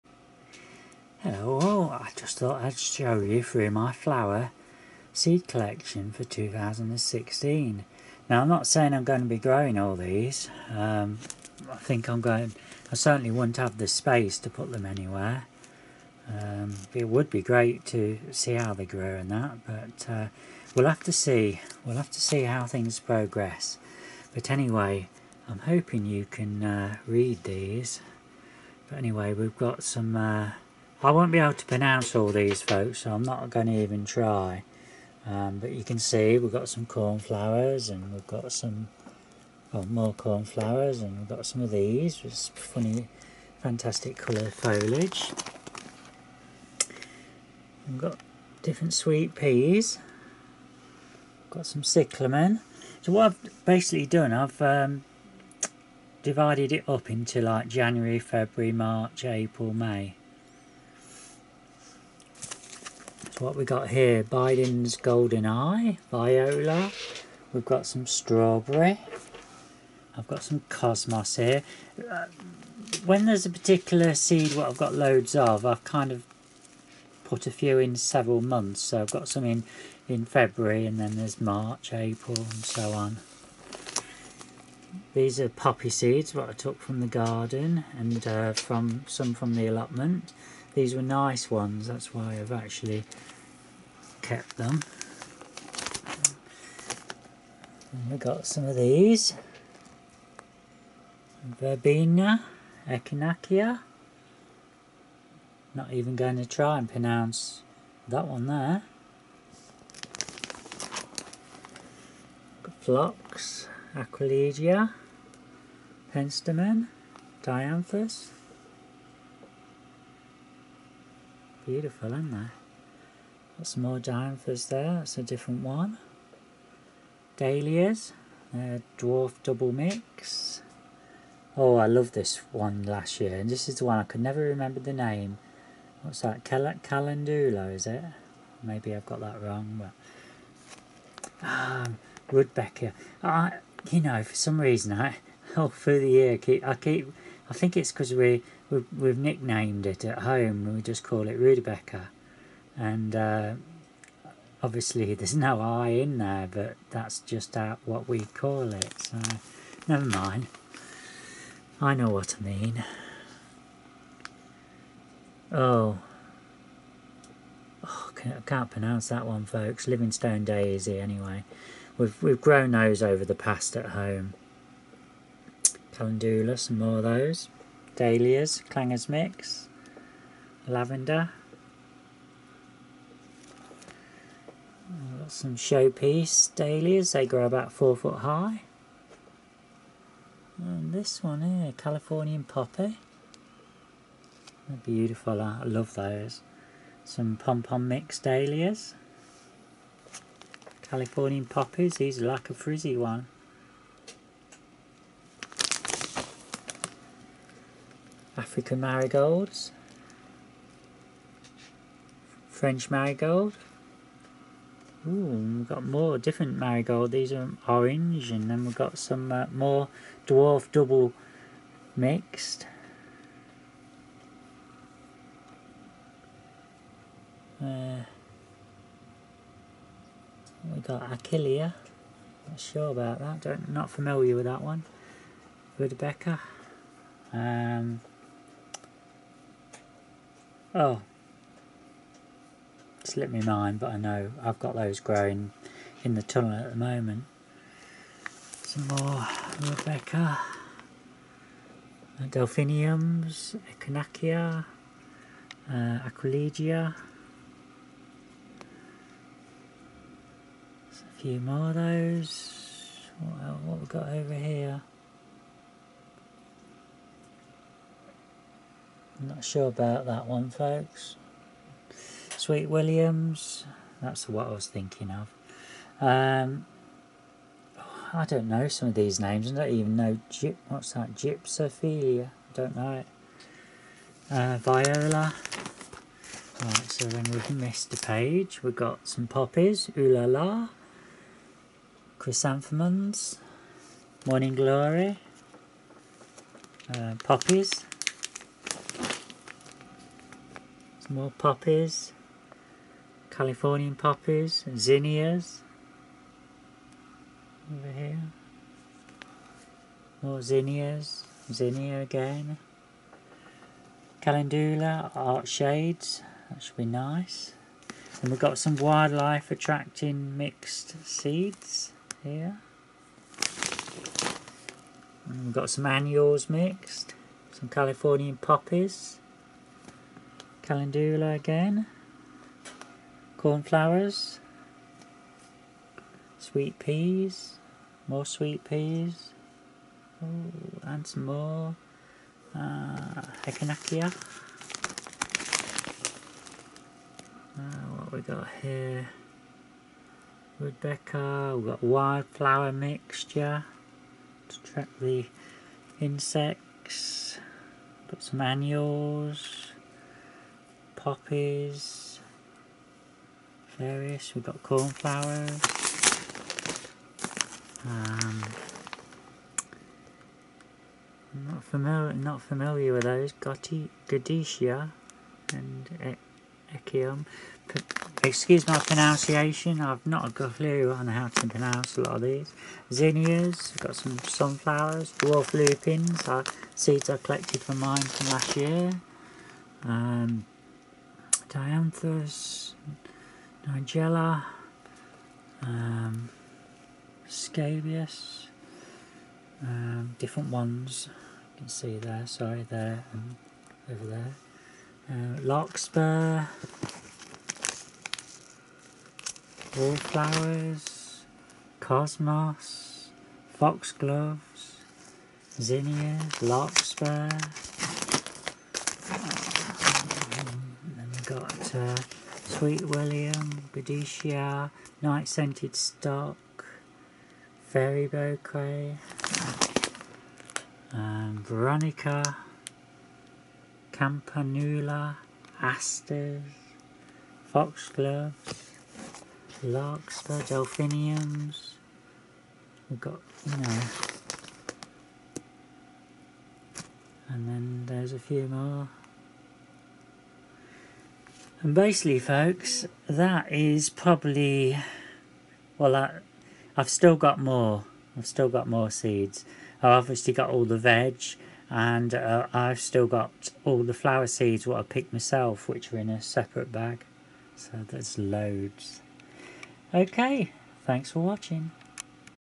Planning for 2016 - Lots of flower seeds
Today I spent a couple of hours going through my seed collection and grouping them by month to sow.  Where I have several packets of a particular seed I have distributed them over several months.  The reason for this is you never know what the weather will throw at us and so better to be safe than sorry.